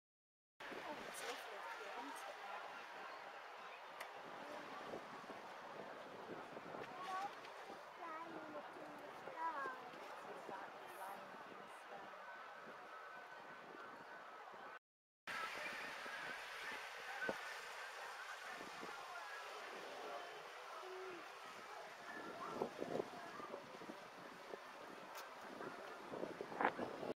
360° VR view from the top of the Lighthouse in Whitby
A view from the top of the lighthouse on the edge of the pier in Whitby.